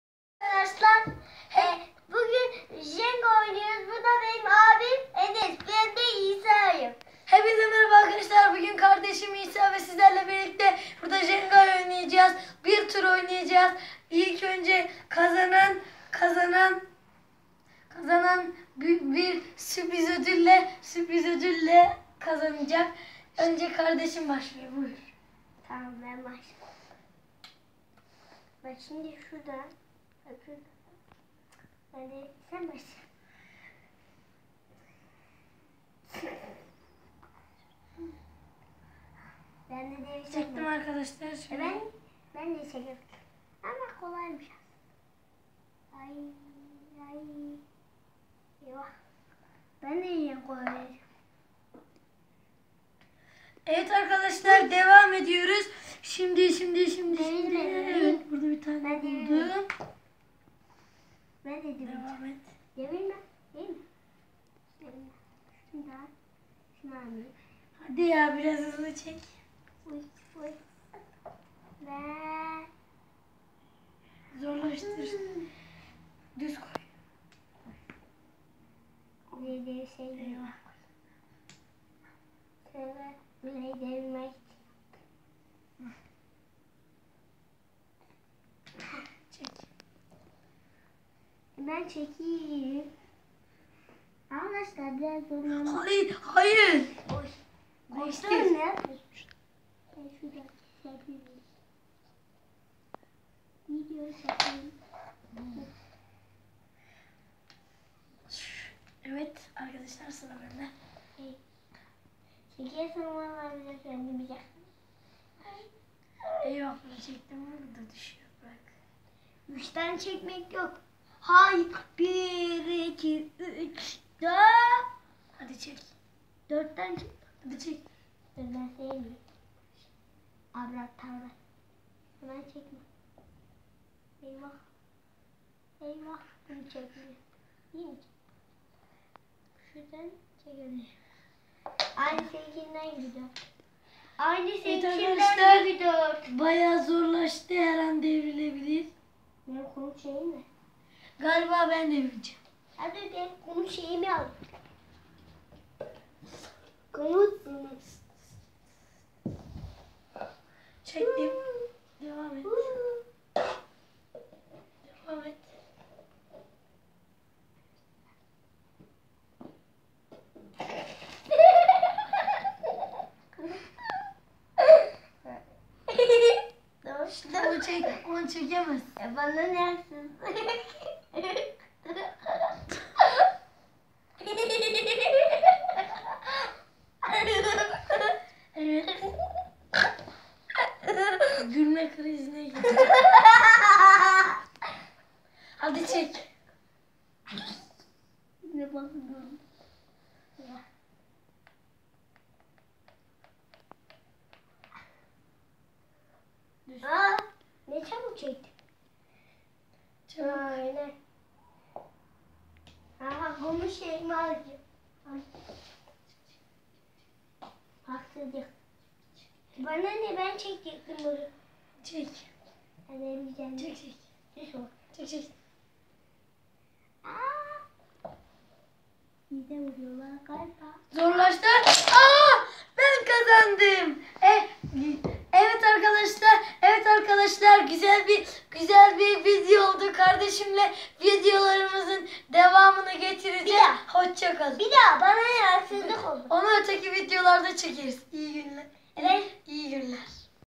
Jengaylar
arkadaslar kardesimle jengay oynadik iyi bir video oldu iyi seyirler